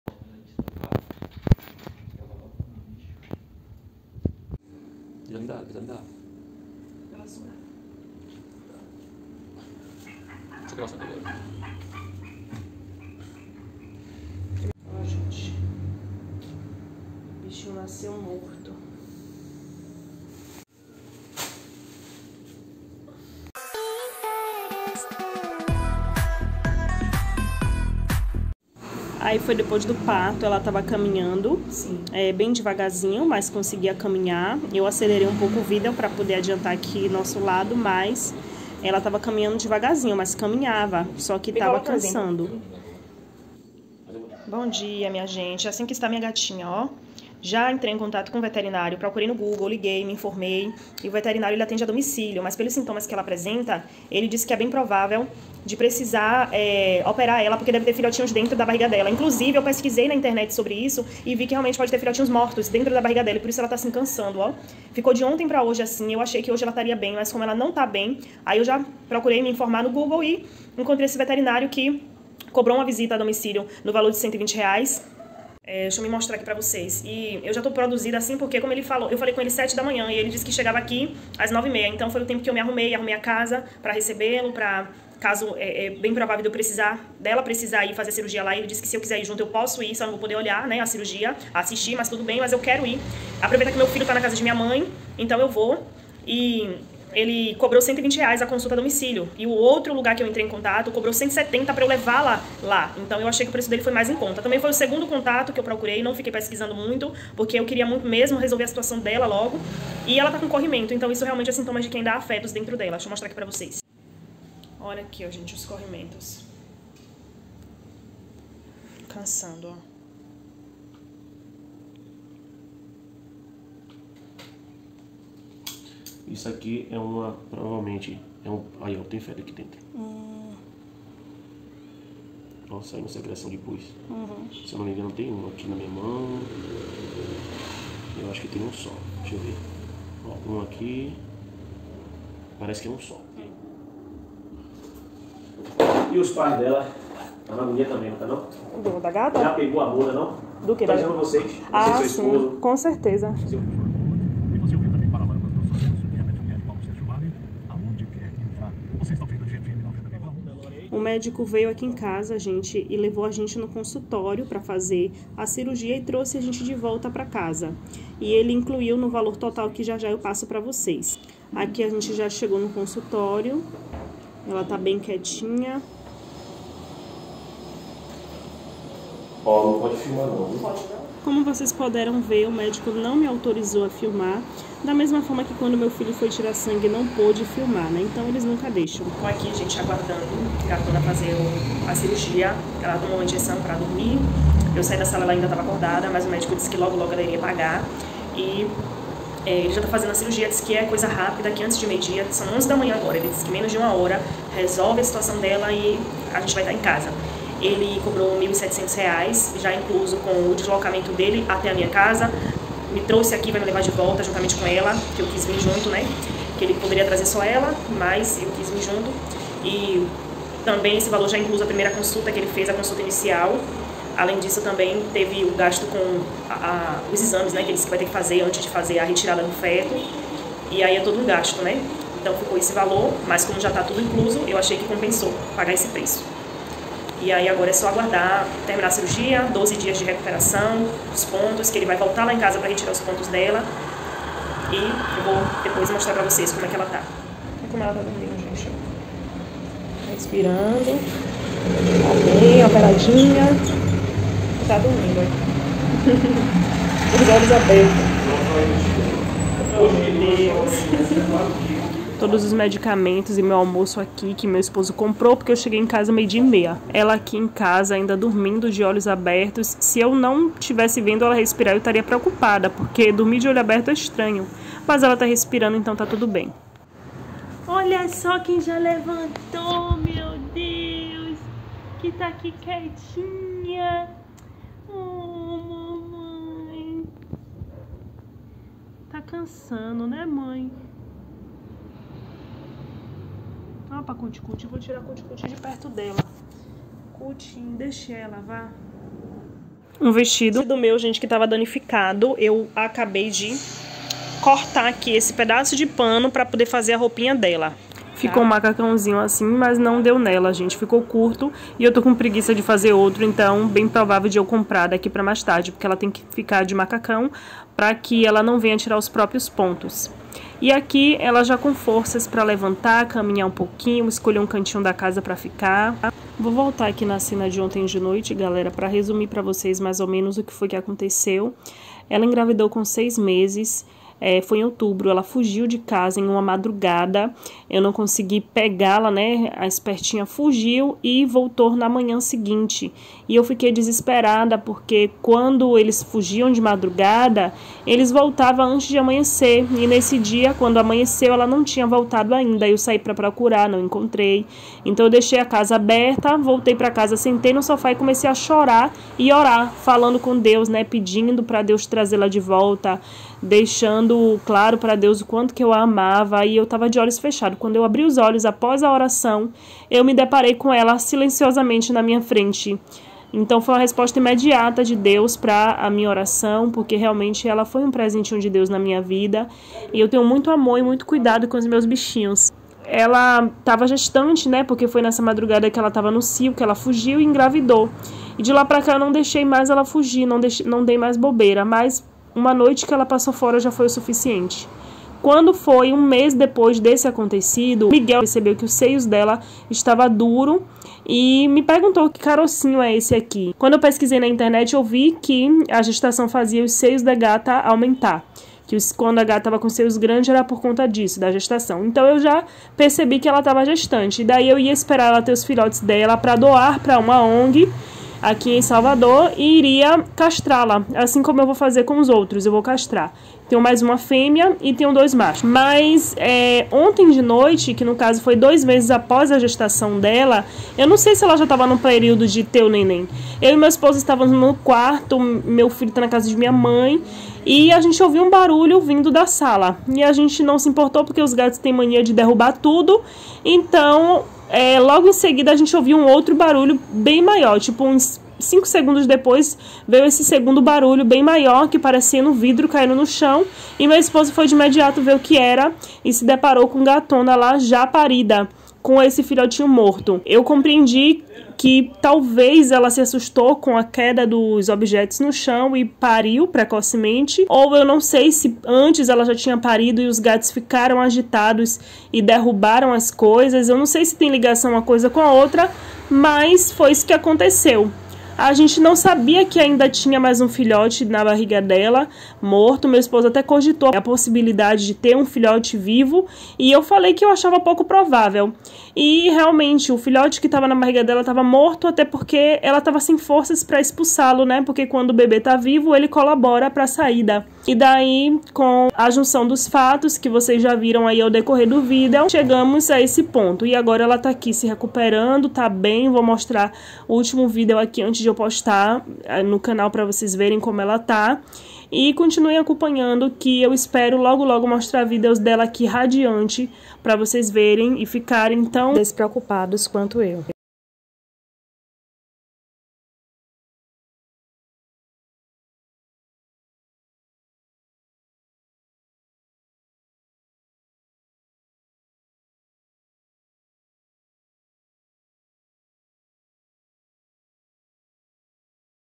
O cirurgião retirou 1 filhote m0rt0 de dentro da minha gata e o outro nasceu m0rt0 no dia anterior
Iaê, minha gente!